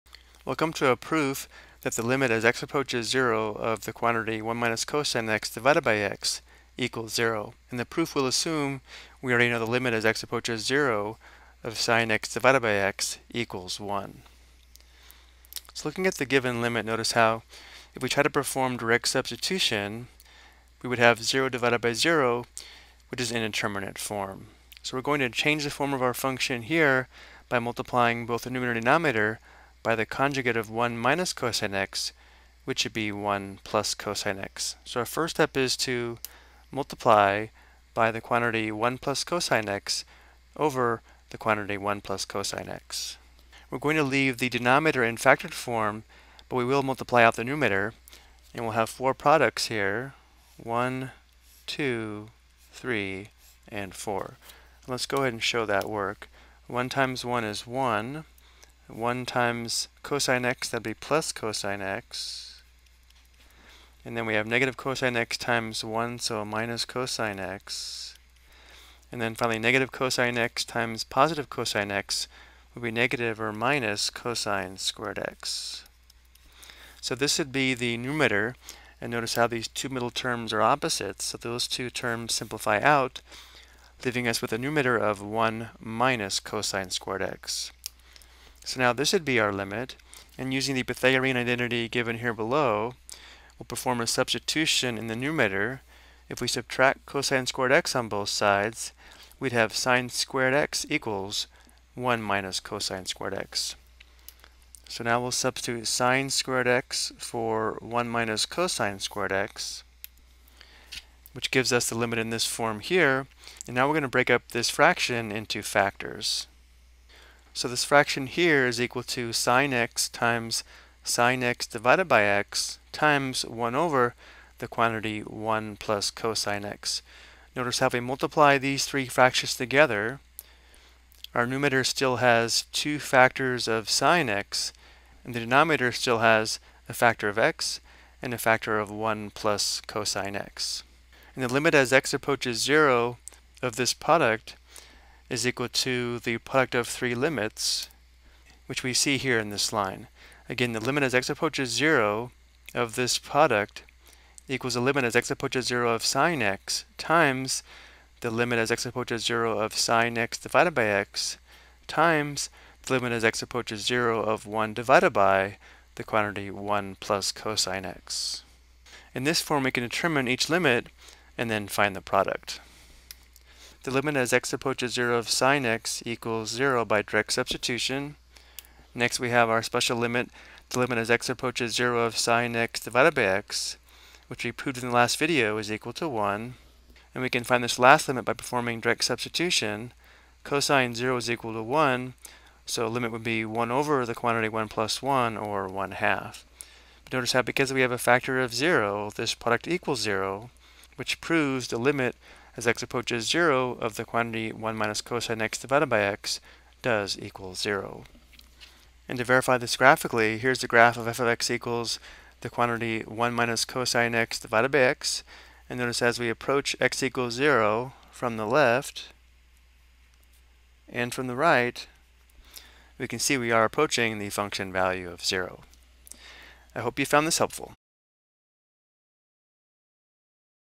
Prove the Limit as x Approaches 0 of (1-cos(x))/x
This video explains how to prove the limit as x approaches 0 of (1-cos(x))/x equals 1 using a trig identity and a special limit involving sin(x)